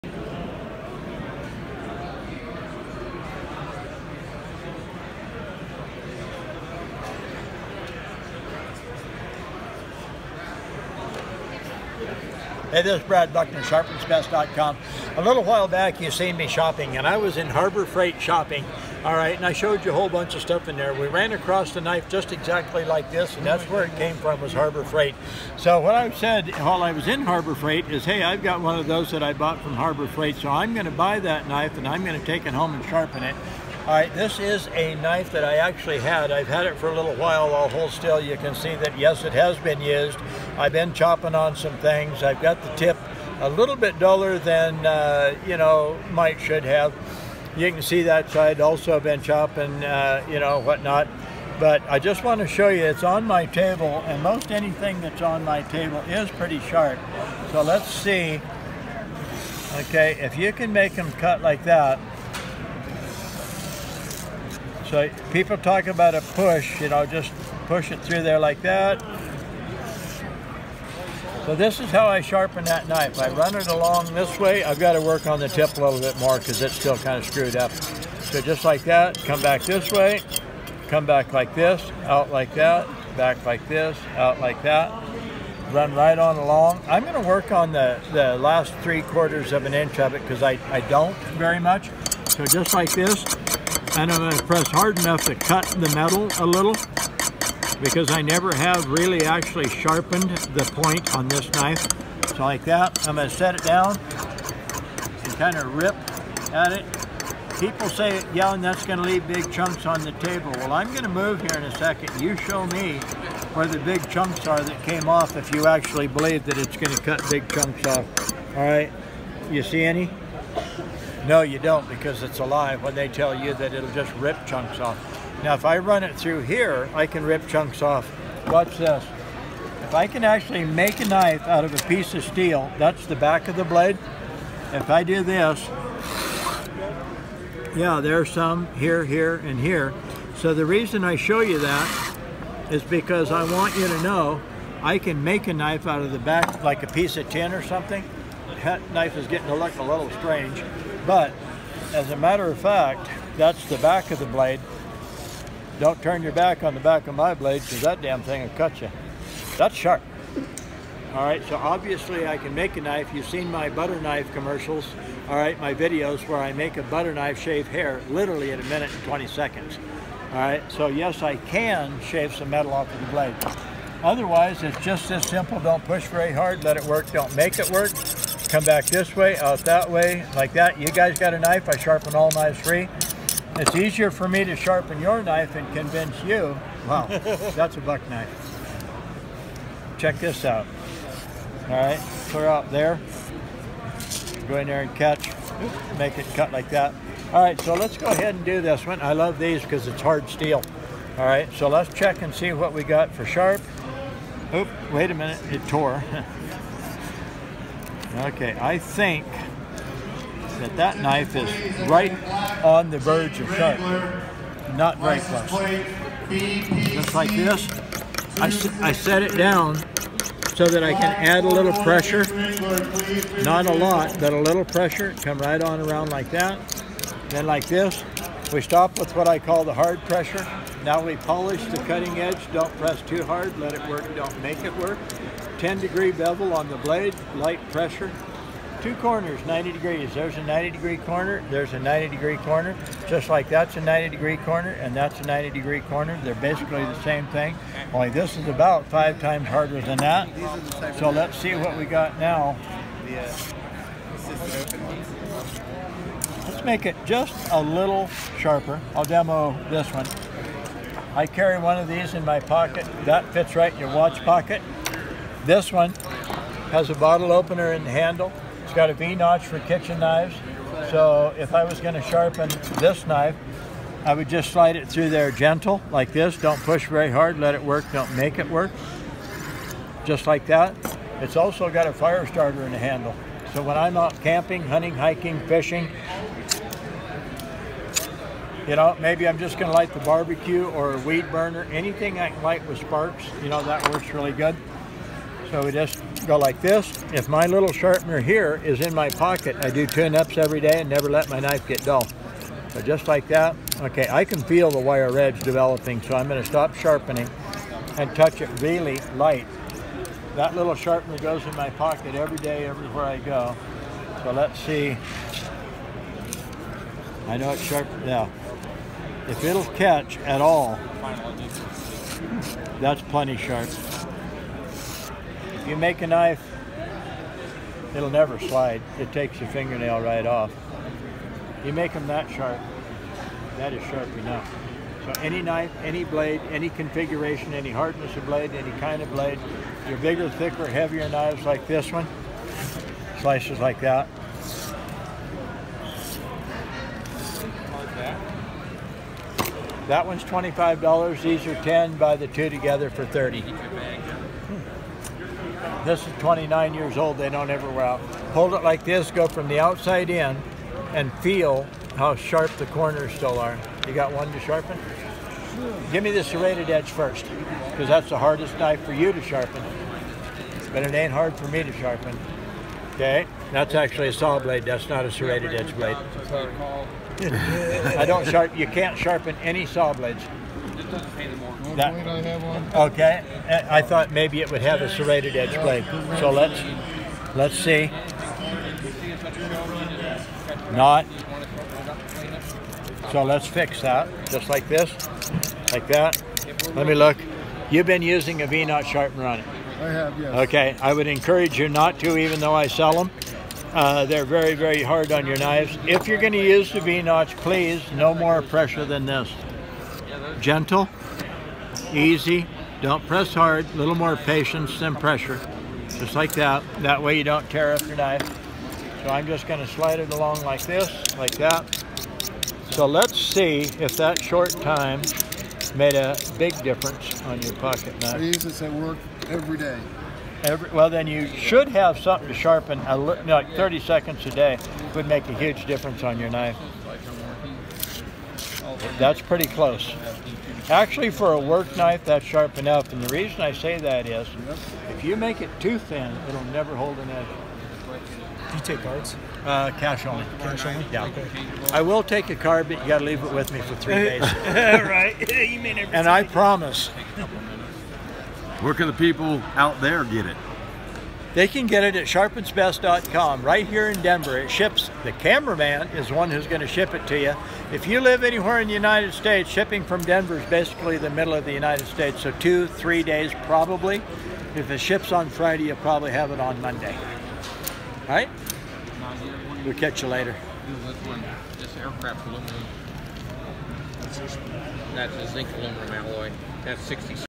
Making Knives SHARP! Harbor Freight’s Sharpest Knife Ever and Demo at the Gun Show
Have you ever seen a sharp knife? Did you know you could make even a Harbor Freight big knife super sharp?
was at the gun show demonstrating how to make this knife and any other knife the sharpest it can be.

Get anything from Harbor Freight, bring it to Brad, and watch him get it super sharp with the Sharpens Best family of knife sharpeners and tool sharpeners.